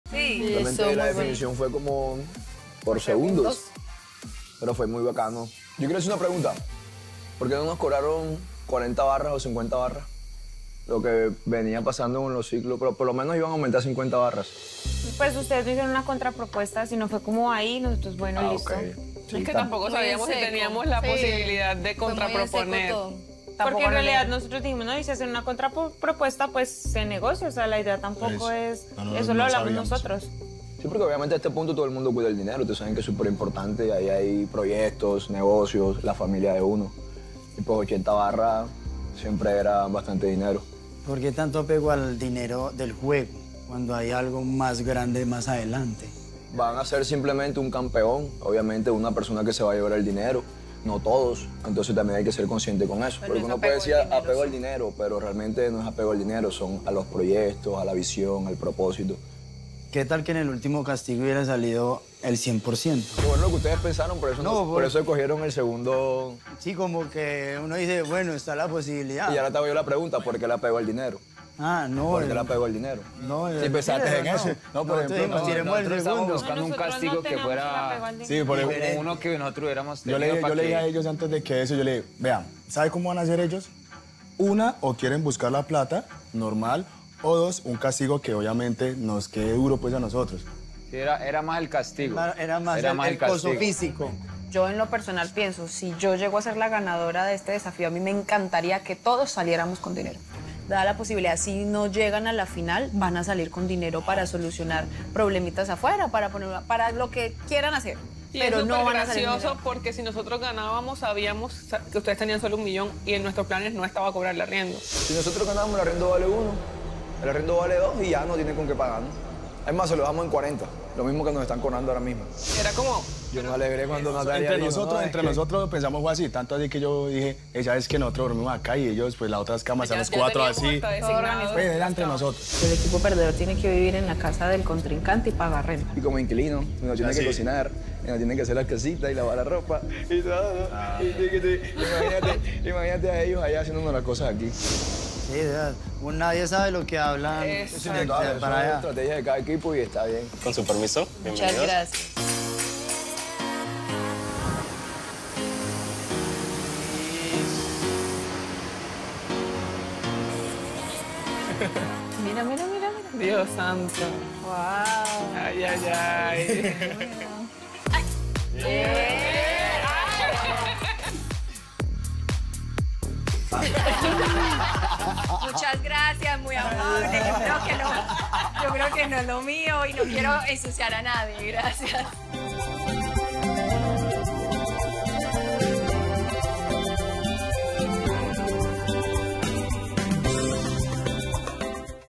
Previo a los Duelos de Salvación los equipos tienen un incómodo momento al hablar de dinero
Jerry y Sensei se enfrentan por su opinión acerca del dinero, pues el equipo Pibe pregunta por qué les cobraron tanto y los del Tino cuestionan por qué no hicieron una contrapropuesta.